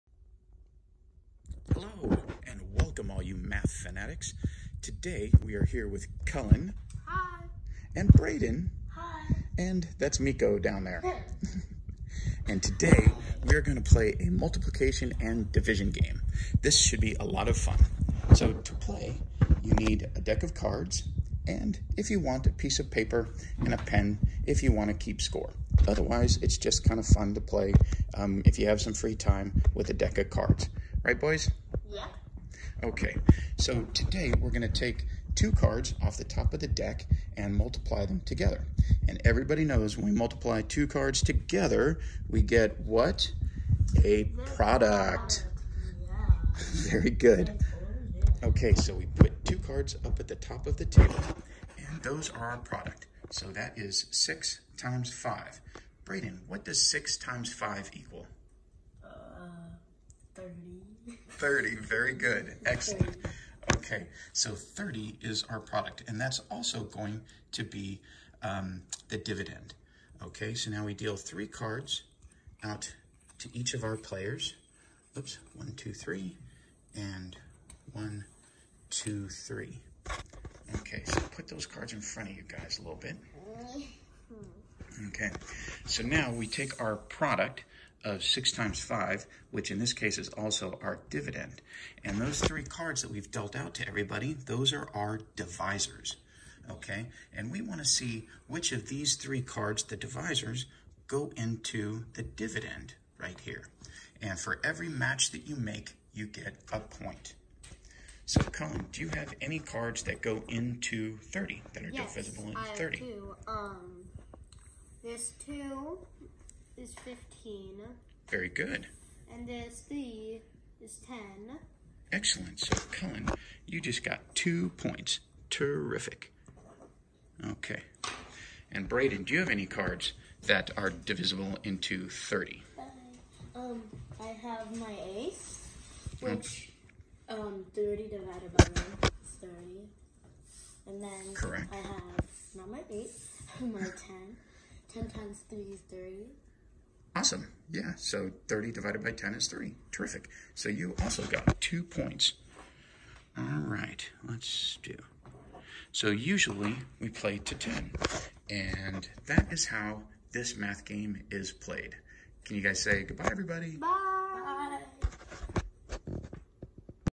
Division Game: Is it divisible?
Division Duel in this game kids identify friendly divisible numbers with a deck of cards.